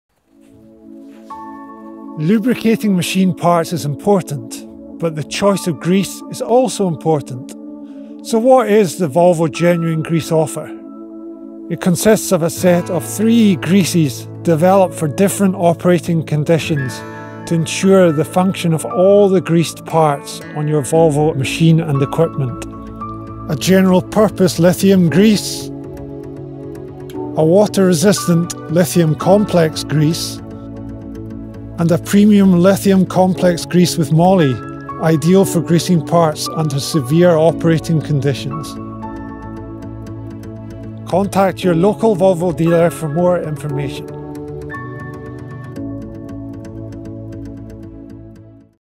Volvo Services Grease Offering: Multi-purpose, Resistant and Extreme greases.
What is the Volvo Genuine Grease offer developed for different operating conditions ?
To ensure the function of all greased parts on your Volvo machines we have a set of 3 different greases:
General Multipurpose Lithium Grease 97718 Gr101,
Water Resistant Resistant Lithium Grease 97720 Gr102,
Premium Extreme Lithium Complex Moly Grease 97765 Gr103.